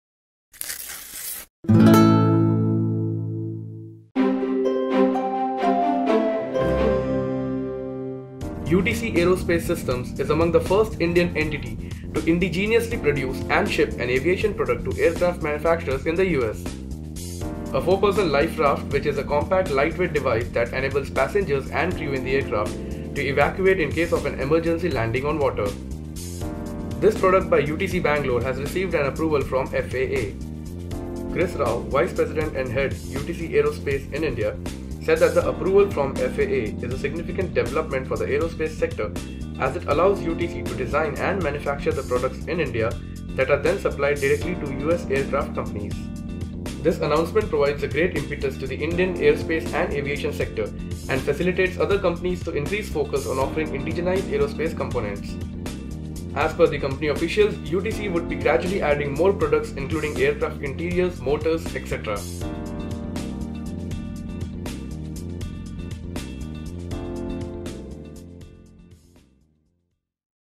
UTC’s India unit gets FAA approval for selling products directly
UTC Aerospace Systems on Tuesday said that US Federal Aviation Administration (FAA) has approved a product developed and produced by the firm in India. UTC Aerospace Systems, a Bangalore based facility is among the first Indian entity to indigenously produce and directly ship an aviation product to aircraft manufacturers in the US.